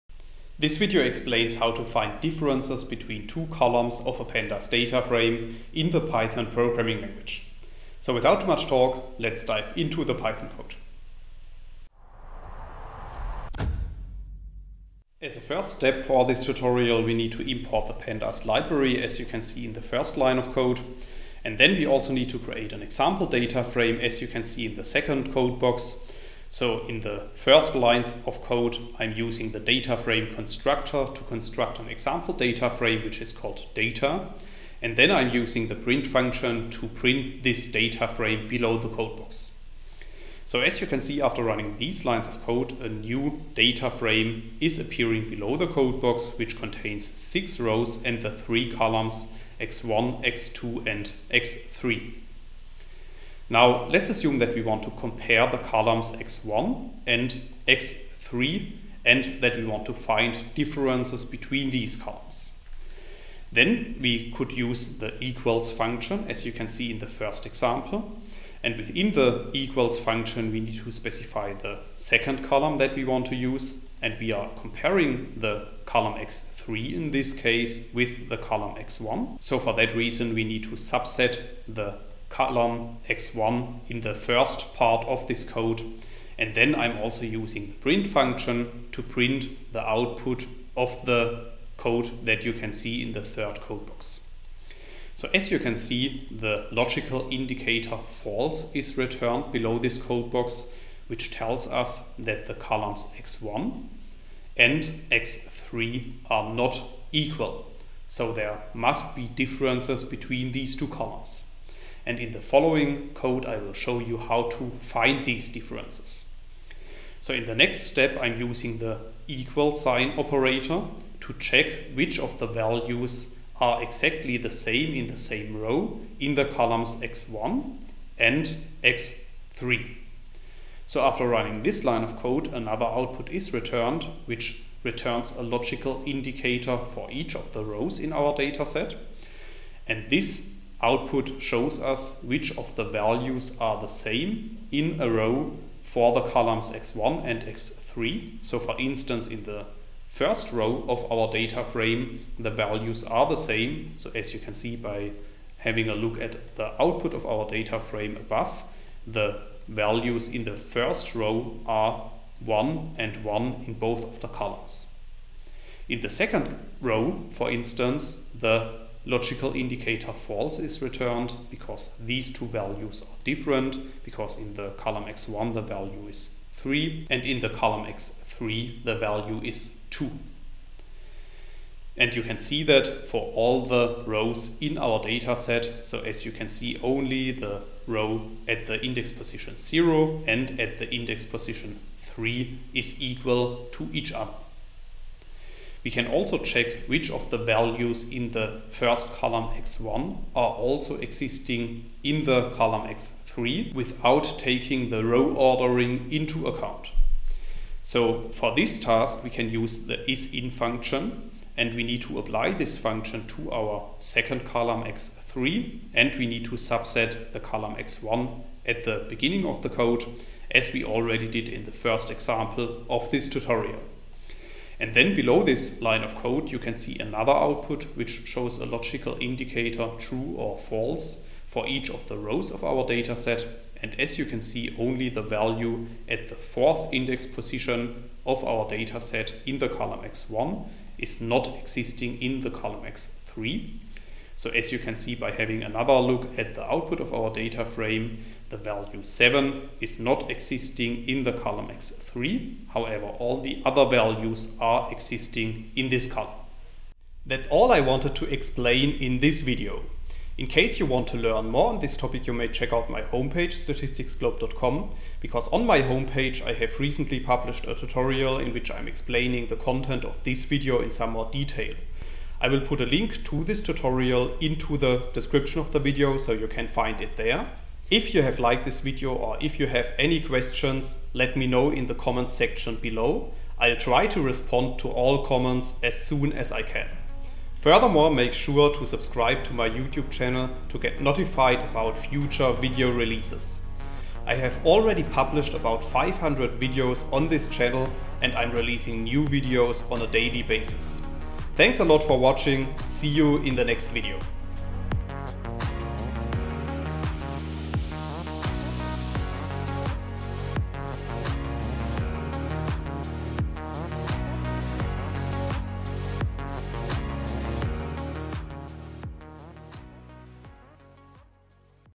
Find Differences Between Two Columns of pandas DataFrame in Python (3 Examples) | equals() Function
How to compare two columns of a pandas DataFrame in the Python programming language. More details
Python code of this video:

import pandas as pd                              # Load pandas

data = pd.DataFrame({'x1':[1, 3, 2, 4, 7, 5],    # Create first pandas DataFrame
                     'x2':['a', 'b', 'c', 'd', 'e', 'f'],
                     'x3':range(1, 7)})
print(data)                                      # Print first pandas DataFrame

print(data['x1'].equals(data['x3']))             # Apply equals function
# False

print(data['x1'] == data['x3'])                  # Apply logical operator
# 0     True
# 1    False
# 2    False
# 3     True
# 4    False
# 5    False
# dtype: bool

print(data['x1'].isin(data['x3']))               # Apply isin function
# 0     True
# 1     True
# 2     True
# 3     True
# 4    False
# 5     True
# Name: x1, dtype: bool